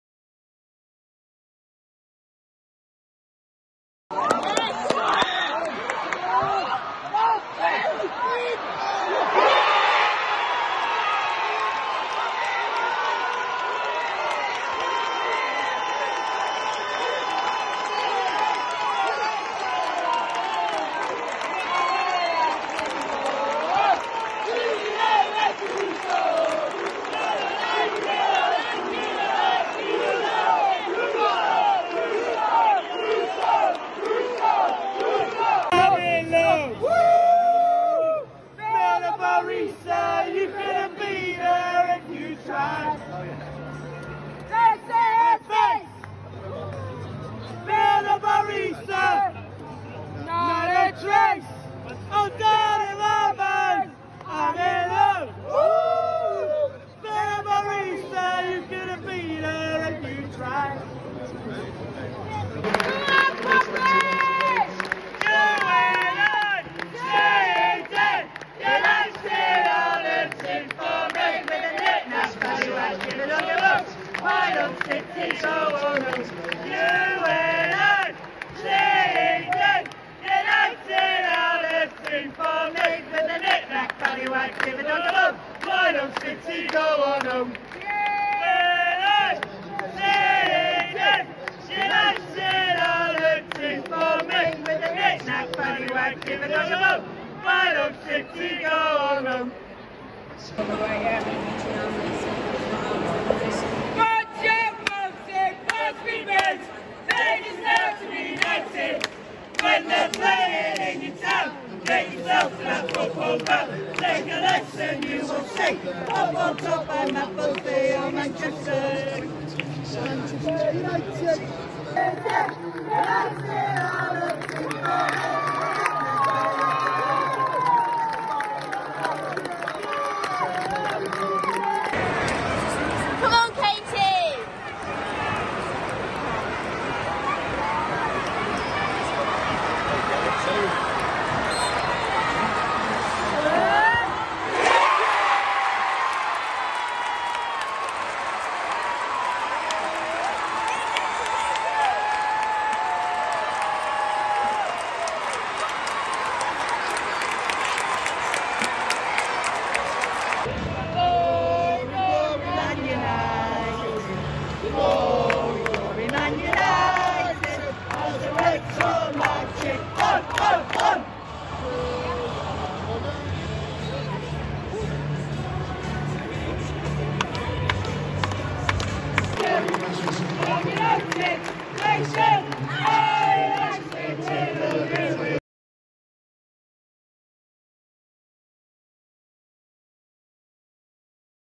Manchester United Women vs Everton Women - Old Trafford Highlights
Match highlights from the stands at Old Trafford during the game between Manchester United Women and Everton Women in the league.

With the first goal from Alessia Russo cancelling out Everton's early shock goal taking us into half time at 1-1. Half time saw the Manchester United Women Under 21s parading the WSL Academy Cup they recently won at St George's Park against Birmingham Under 21s.

The second half saw a Katie Zelem penalty to put United 2-1 in the lead. Controversially, a Martha Thomas goal was ruled out incorrectly for offside before Alessia Russo sealed the win with a headed goal in front of the Stretford End.

First Goal Clip Credit: Karen Blyth
Penalty Clip Credit: Jessica Sartain